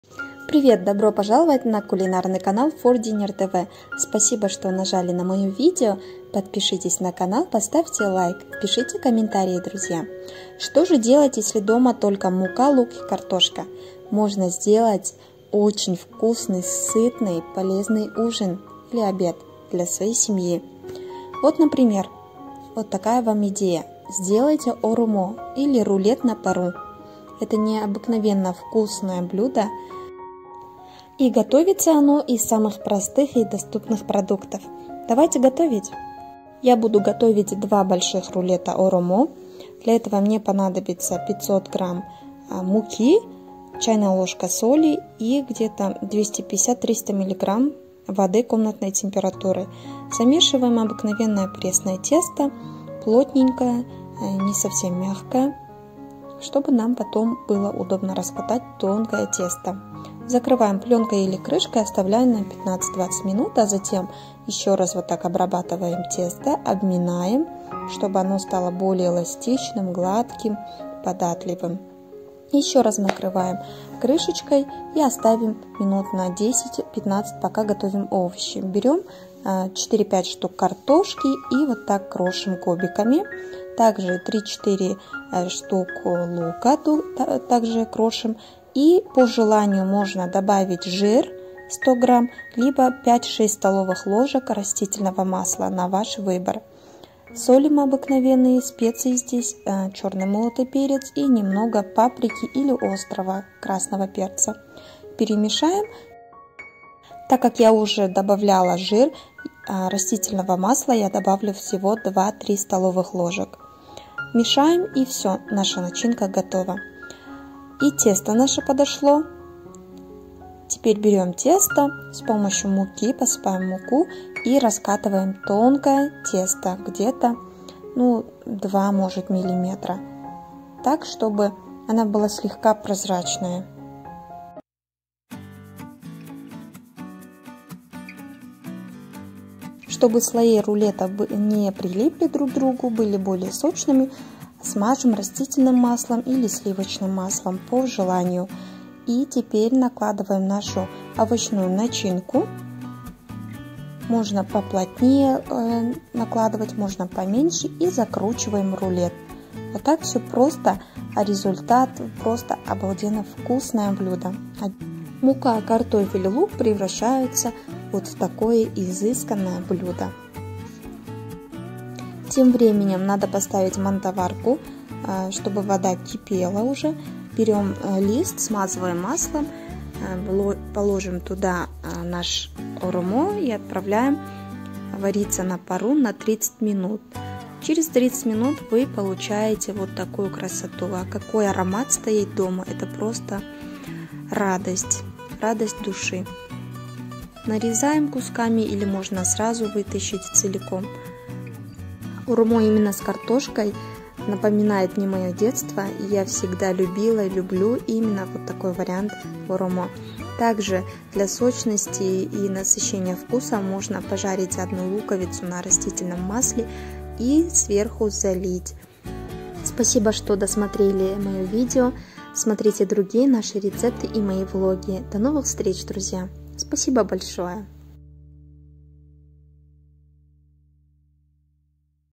Всего 3 продукта😳 ОРОМО/ Ханум/ Рулет на пару/БЮДЖЕТНО И ВКУСНО
Рецепт Оромо, Ханум,Рулет на пару

1 оромо, рулет= 3,4 порции.
На 1 оромо необходимо 250 гр муки, пол чайной ложки соли, 110 мл воды.

для начинки на 1 оромо:
 2 больших картофеля и 2 больших лука, 2 столовых ложек растительного масла (жир , курдюк, 50 гр по желанию) соль, черный молотый перец, паприка по пол чайной ложки.

Умножайте эти пропорции на столько, сколько штук оромо,рулета хотите сделать.

Варим на пару 30 минут.
подаем с айрном, йогуртом, аджикой.